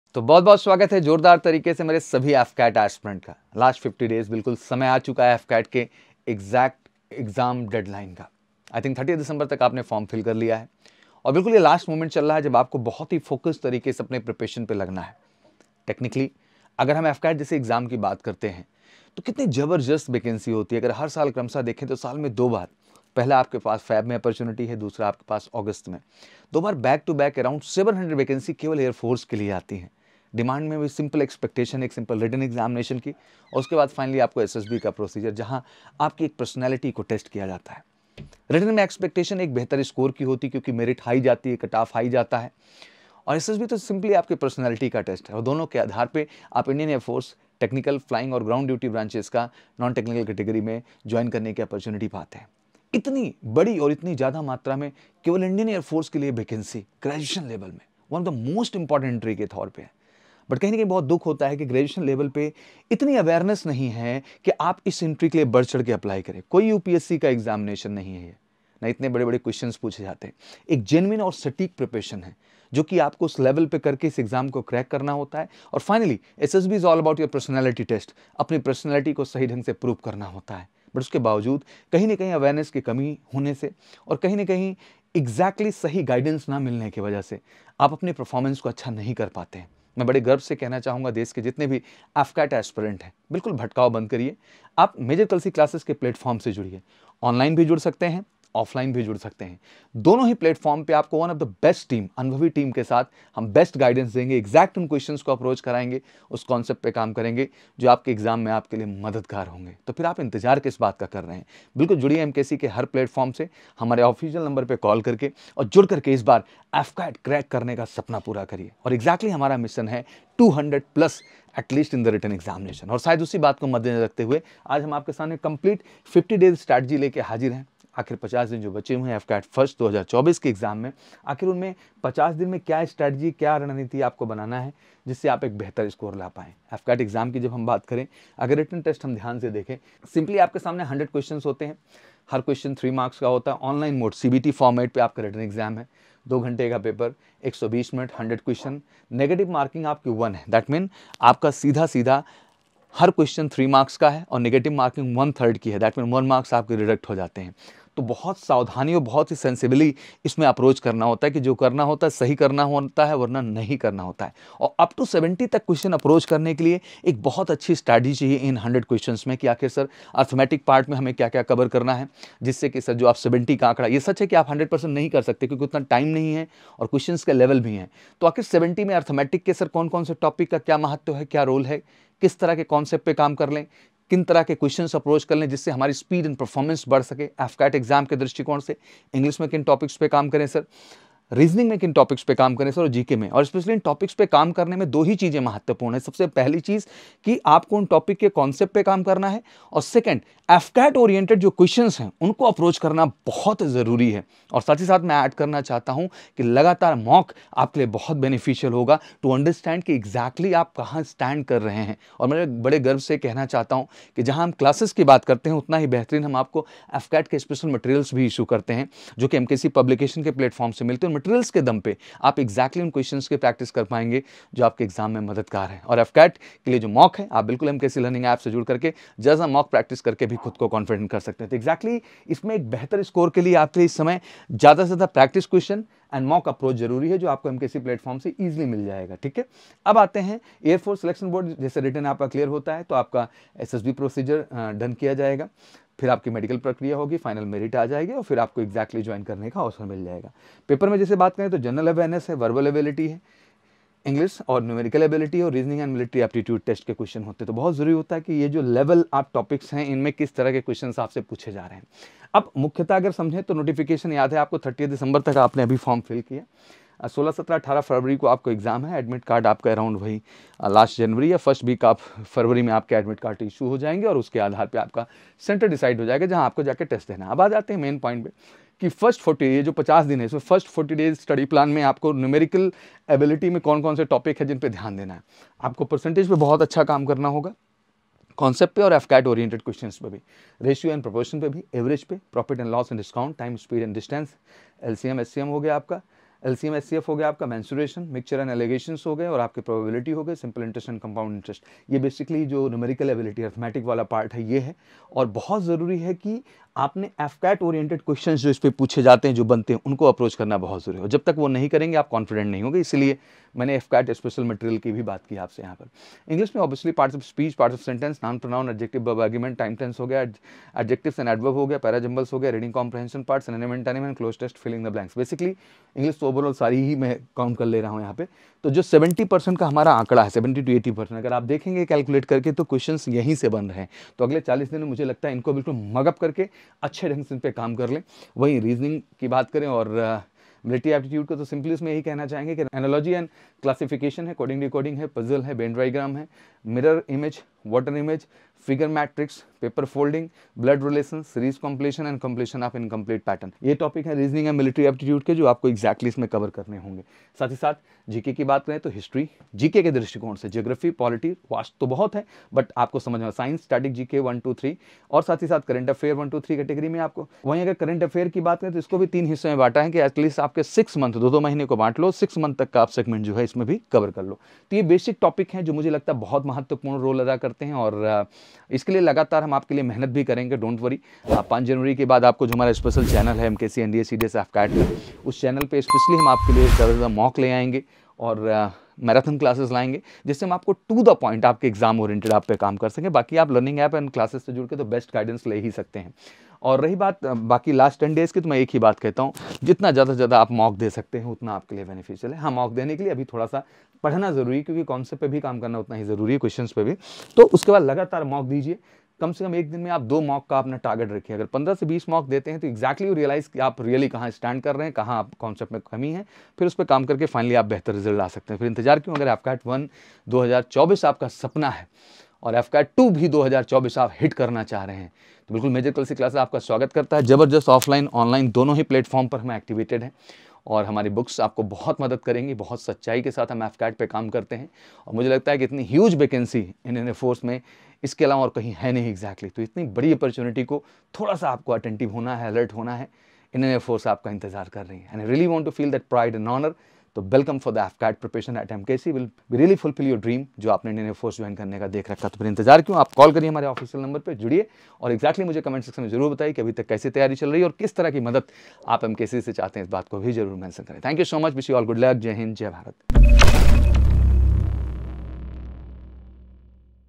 AFCAT 1 2024 Exam | 50 Days Strategy to Clear AFCAT 1 2024 Exam | AFCAT 2024 Preparation Strategy
🛫 AFCAT 1 2024 Exam - Your 50-Day Roadmap to Success! 🛫

Are you gearing up for the AFCAT 1 2024 Exam? Dive into this comprehensive guide tailored by the expert, Saurabh Sir, to strategize your preparation effectively and ace the exam with confidence.

📌 Video Highlights:

📆 50-Day Action Plan: Saurabh Sir unfolds a meticulously crafted 50-day strategy, ensuring every aspirant is well-equipped to tackle the AFCAT 1 2024 Exam head-on.
📘 Subject-wise Breakdown: Gain insights into prioritizing subjects, mastering key concepts, and optimizing your study schedule for maximum productivity.
🎯 Tips & Tricks: Uncover invaluable tips, shortcuts, and best practices that have proven successful for numerous AFCAT aspirants.
🤝 Exclusive Guidance by MKC: Benefit from the collective expertise of MKC in guiding aspirants towards their dream career in the Indian Air Force.
Embark on this transformative journey with us and elevate your AFCAT 2024 preparation to new heights!

Register for 👇
Offline Classroom OR Online Classroom Course